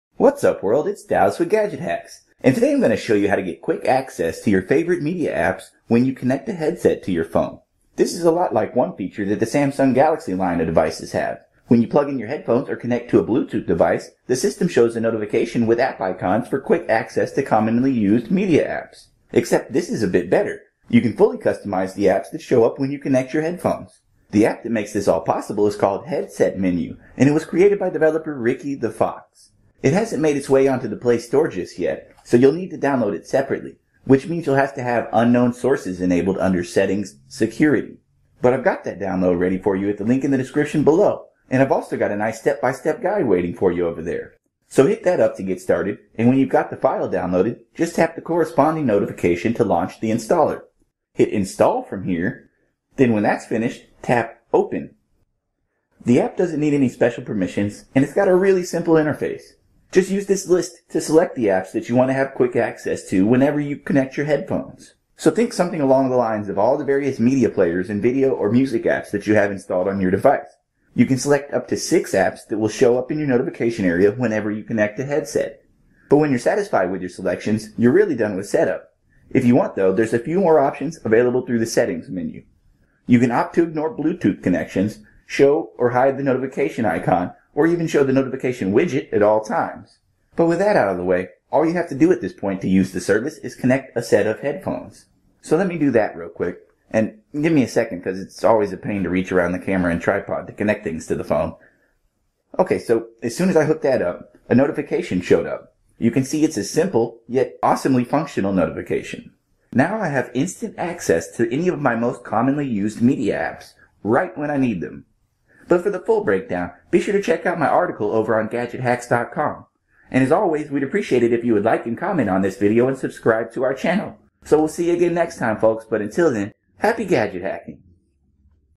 Get Instant Access to Media Apps When You Connect a Headset - Android [How-To]
Quickly Launch Your Favorite Apps on Headset Connect

In this tutorial, I'll be showing you how to get instant access to your most commonly-used media apps as soon as you connect a set of headphones to your device.

Much like a Samsung TouchWiz feature, the app "Headset Menu" automatically shows a notification widget with up to 6 user-set apps as soon as you connect a wired or bluetooth headset.